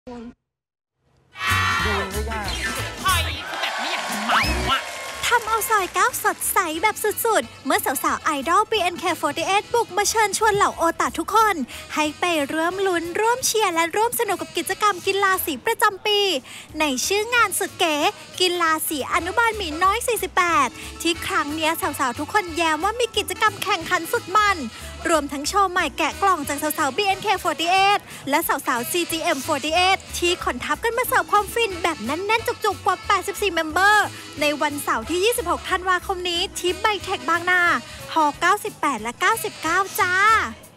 เมาท์มอยซอย 9 : “BNK48” ชวนร่วมฟินในงานกีฬาสีประจำปี
สาว ๆ ไอดอลกรุ๊ป “BNK48” บุกมาเชิญชวนเหล่าโอตะทุกคน ให้ไปร่วมลุ้น ร่วมเชียร์ และร่วมสนุกกับกิจกรรมกีฬาสีประจำปี ในชื่องานสุดเก๋ “กีฬาสีอนุบาลหนูน้อย 48” ที่ครั้งนี้สาว ๆ ทุกคนแย้มว่ามีกิจกรรมแข่งขันสุดมัน รวมทั้งโชว์สดใหม่แกะกล่องจากสาว ๆ BNK48 และสาว ๆ CGM48 ที่ขนทัพมาเสิร์ฟความฟินแบบแน่น ๆ กว่า 84 เมมเบอร์  ในวันเสาร์ที่ 26 ธันวาคม 2563